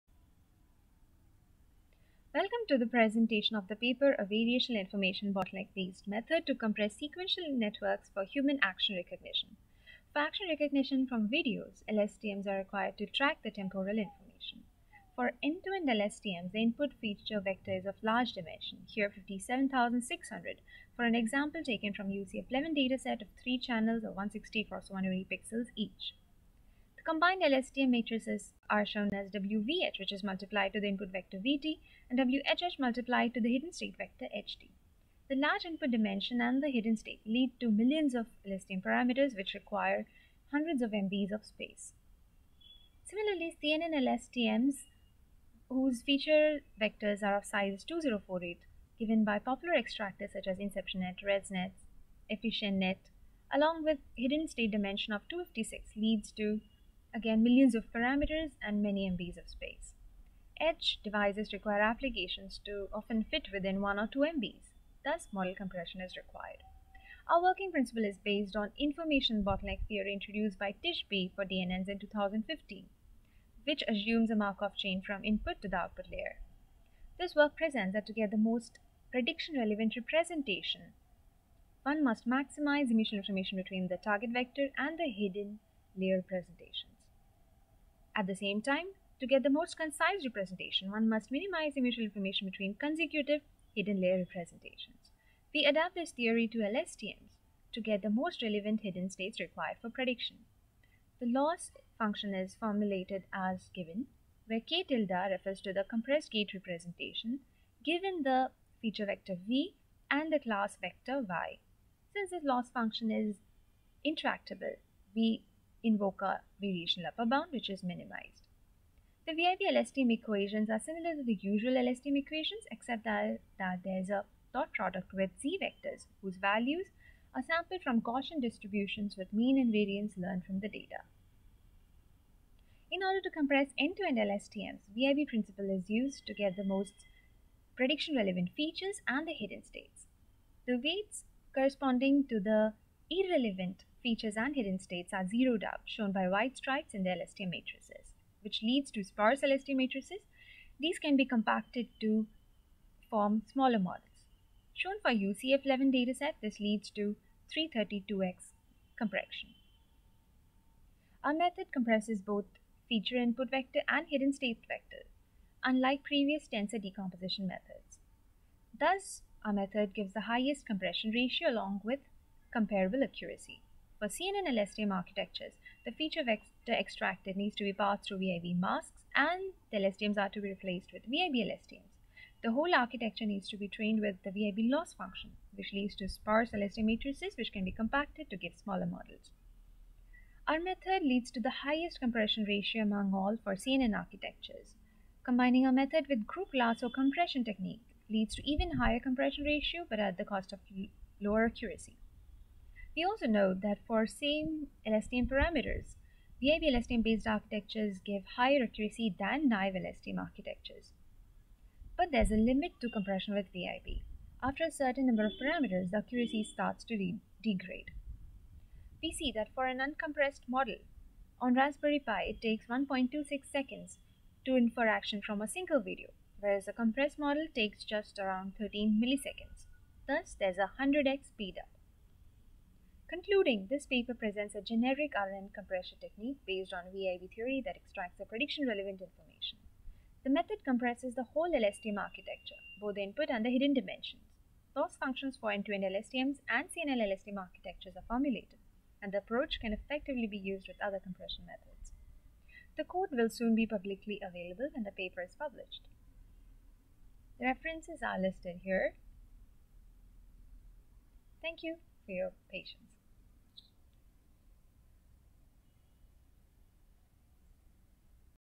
WACV 21 - A Variational Information Bottleneck Based Method to Compress Sequential Networks
Video of presentation of paper "A Variational Information Bottleneck Based Method to Compress Sequential Networks" for Human Action Recognition on  Winter Conference on Applications of Computer Vision (WACV) 2021.

Authors: Oshin Dutta, Ayush Srivastava, Jigyasa Gupta, Sumeet Agarwal, and Prathosh AP.

Indian Institute of Technology, Delhi